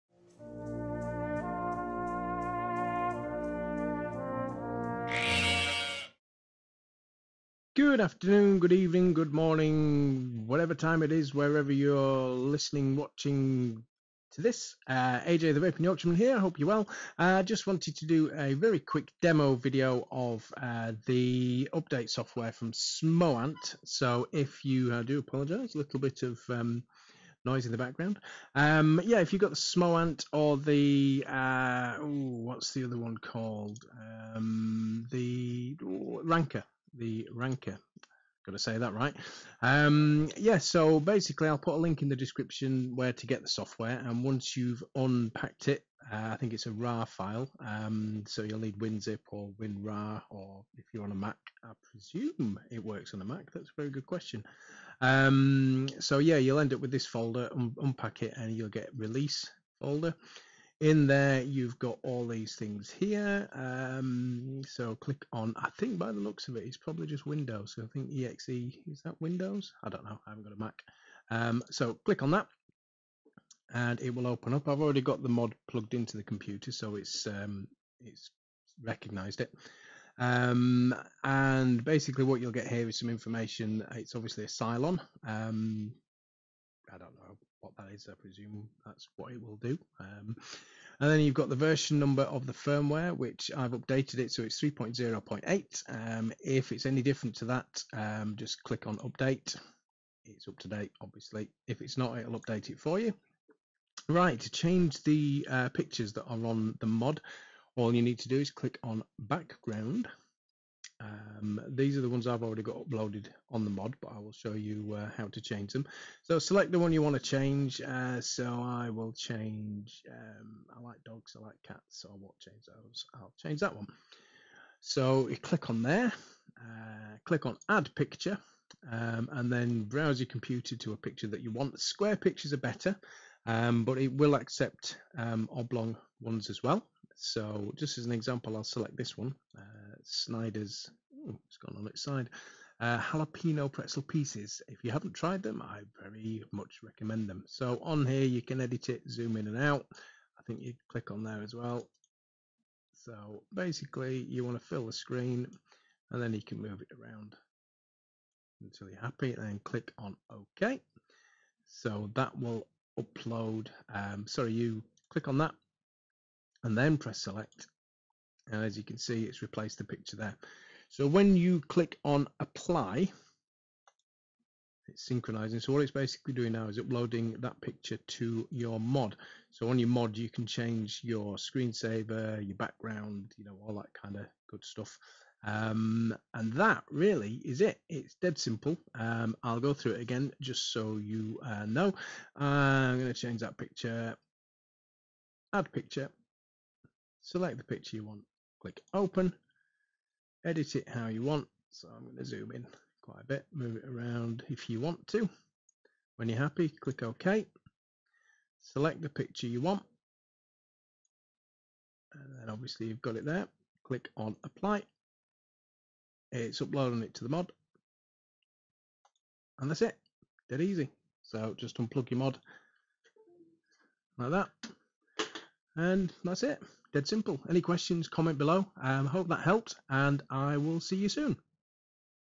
Smoant Firmware and Wallpaper/Screensaver Update
A quick demo of how to upload new screensavers/wallpaper to your Smoant Cylon/Ranker mod.  I think it's only Windows compatible, but I can't say for sure as I don't own a posh Mac!